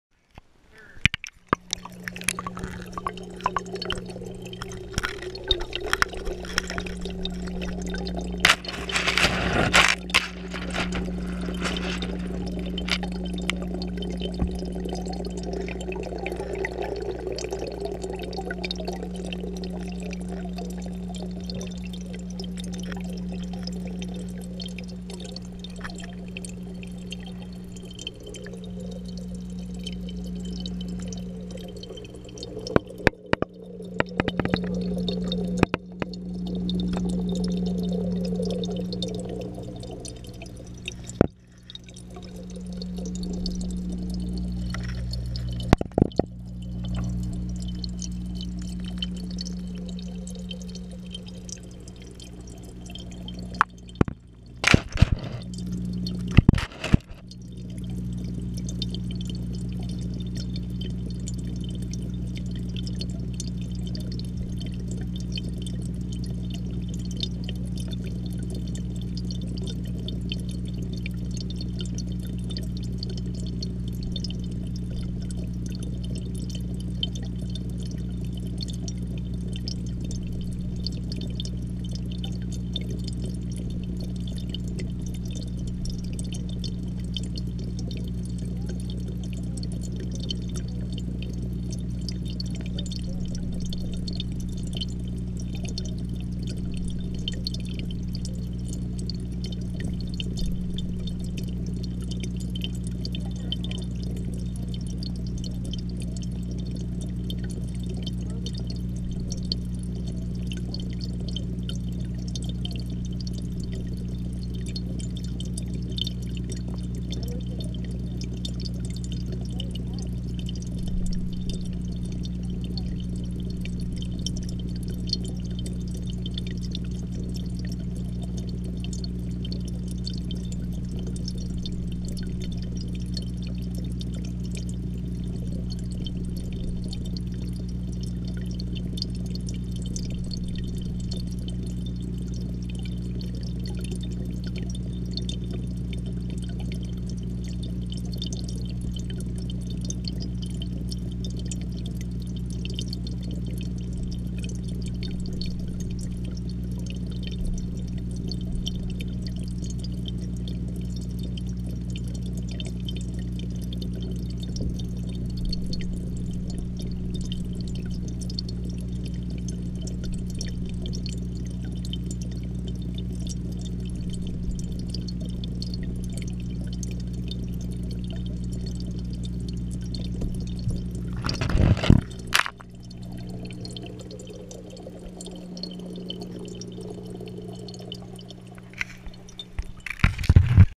GOPRO in fish tank
I put my gopro in my fish tank to test the water proof and how clear it is